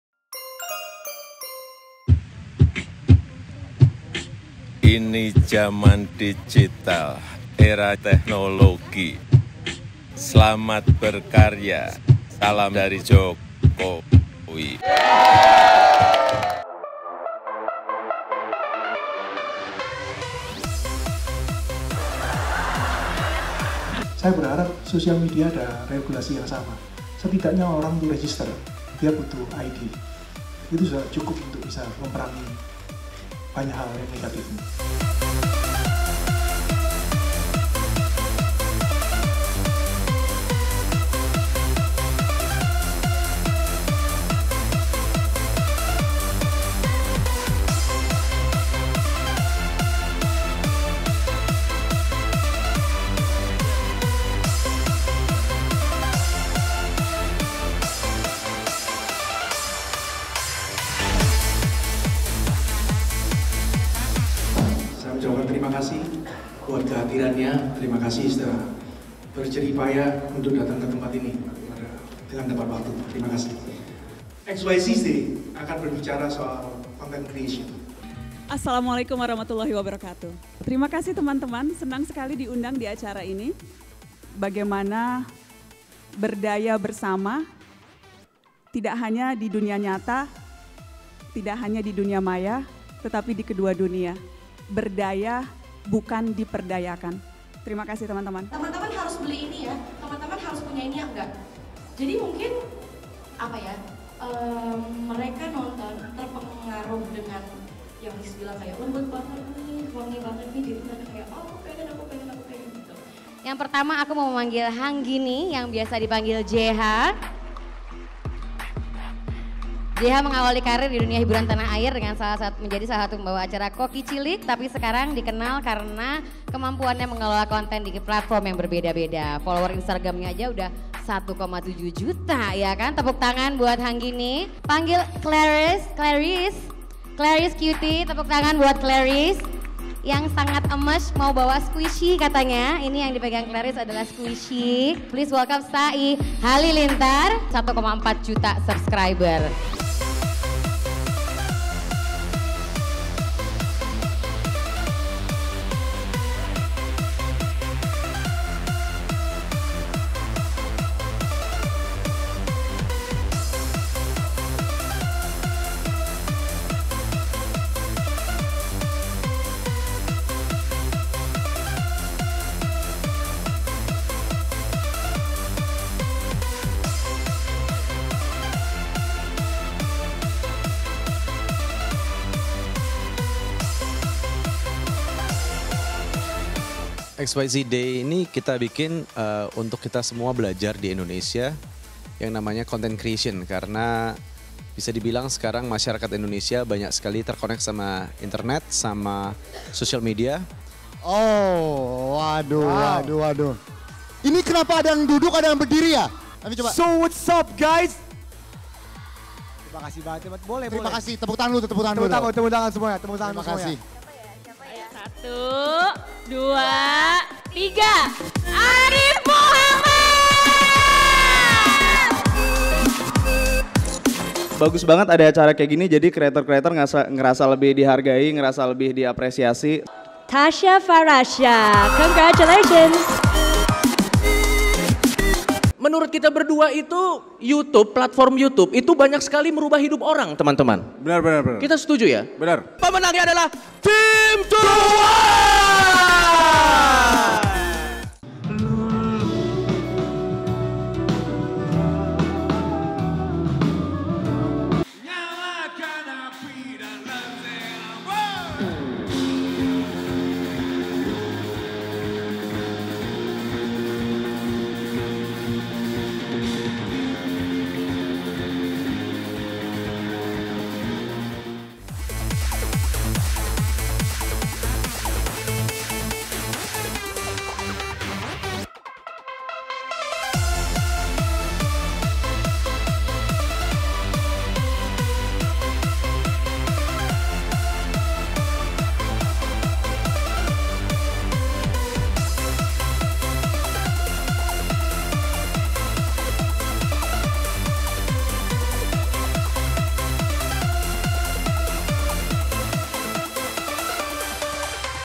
XYZ DAY 2018 - Dari Jokowi, Deddy Corbuzier Hingga Ria Ricis
KAPANLAGI.COM - Hi KLovers! Inilah dia XYZ DAY 2018 yang dilaksanakan pada tanggal 25 April 2018 lalu sangat menyenangkan. Selain mendapat dukungan dari Presiden Jokowi, acara ini juga mempertemukan tiga generasi berbeda dan saling berbagi. Semoga banyak kreator baru yang lahir dari acara ini, ya! Simak yuk serunya di video berikut ini...